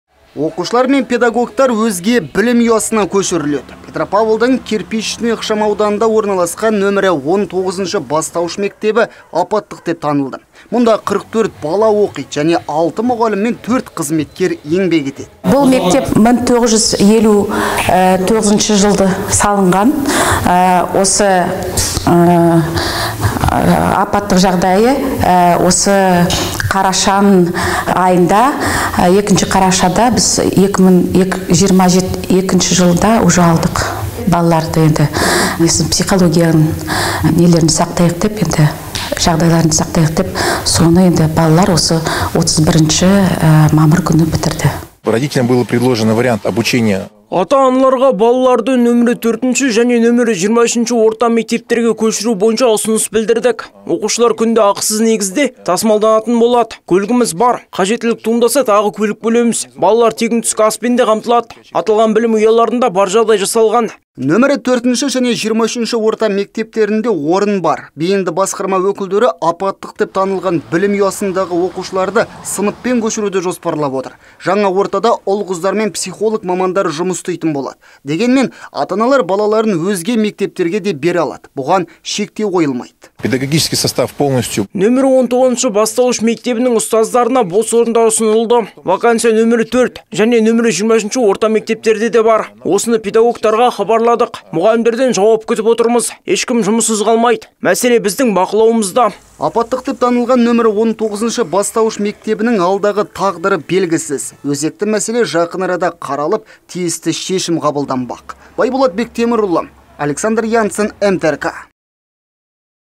Апаттық деп танылған мектептің оқушылары өзге мектепке көшіріледі
Оқушылар мен педагогтар өзге білім ұясына көшіріледі. Петропавлдың Кирпичный ықшам ауданында орналасқан №19-шы бастауыш мектебі апаттық деп танылды. Мұнда 44 бала оқиды және 6 мұғалім мен 4 қызметкер еңбек етеді.